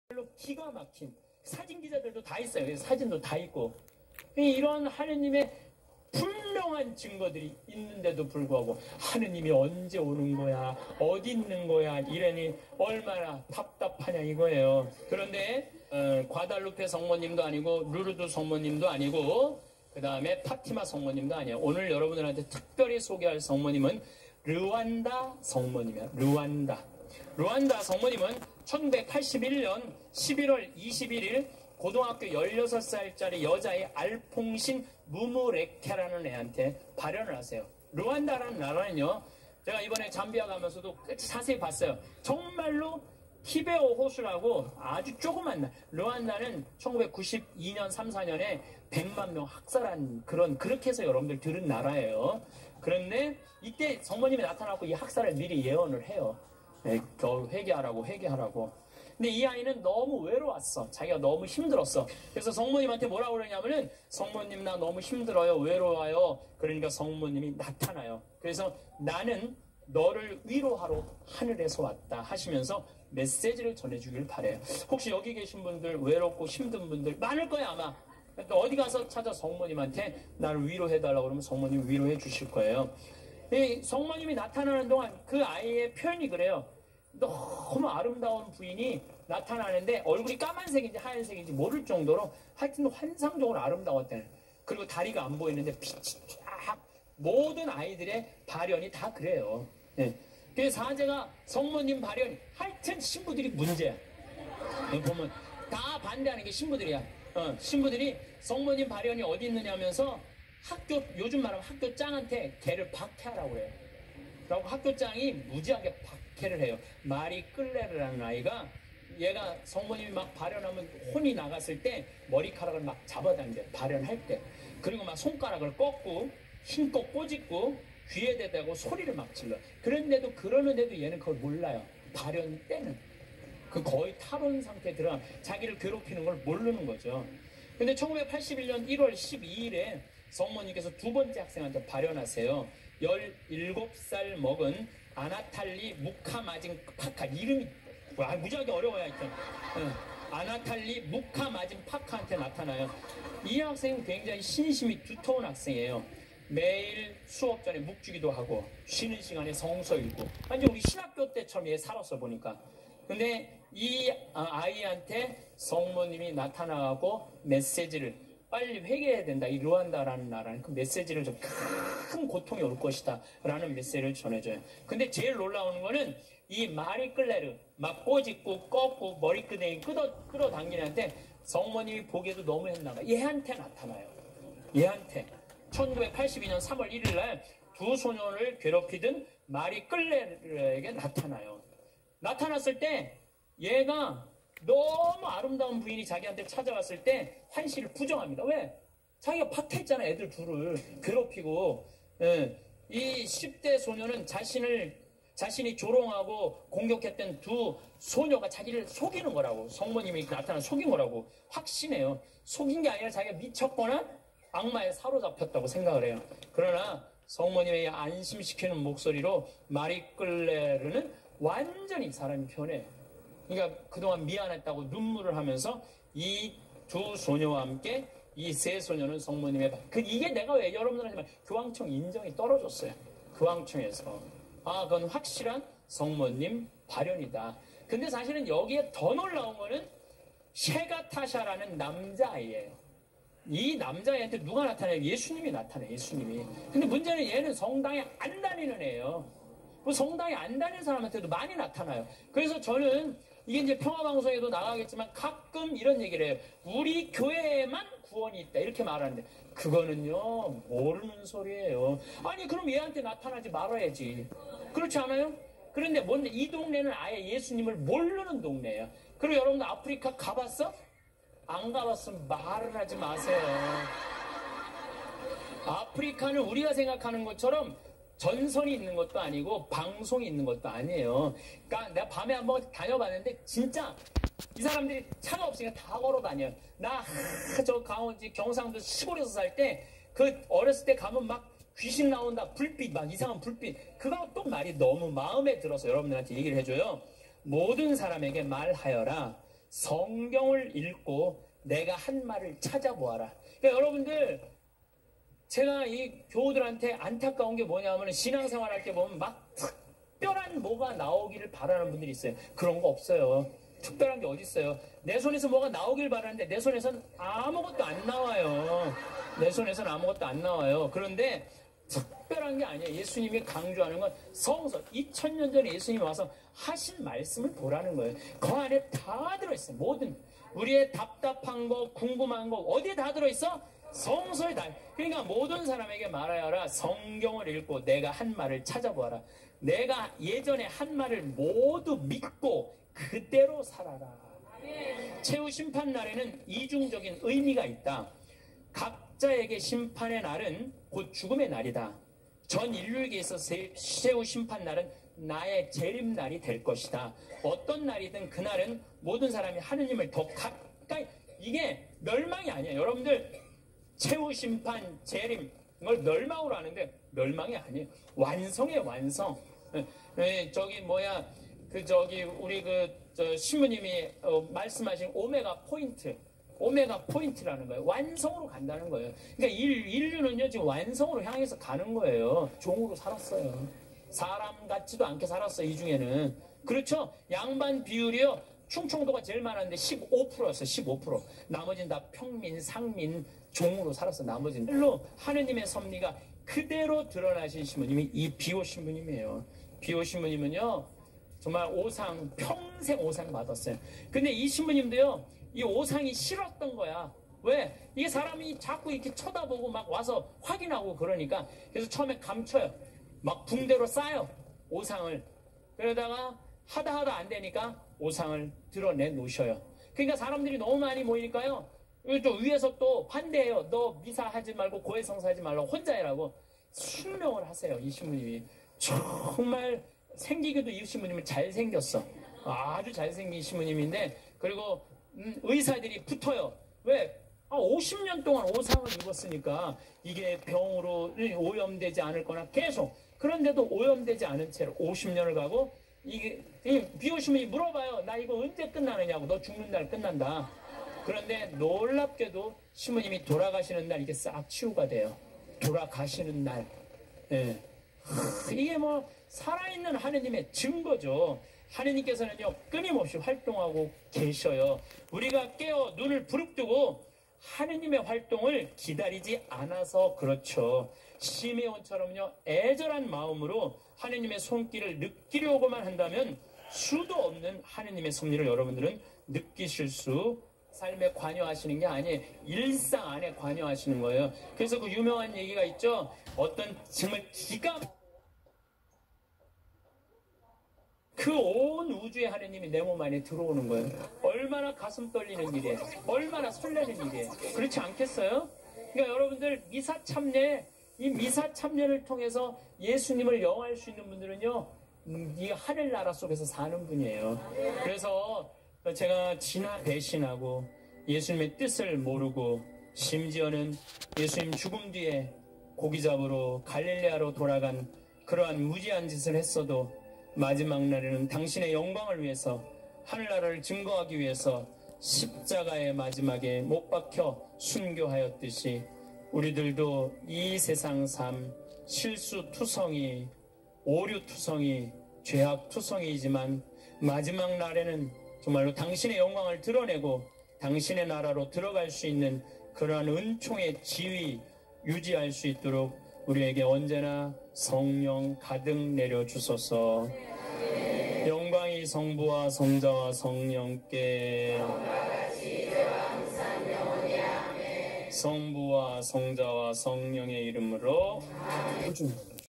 Appearance of St. Maria's apparition in Rwanda
르완다에서  성모마리아 발현  by황창연신부님의 강론   2020년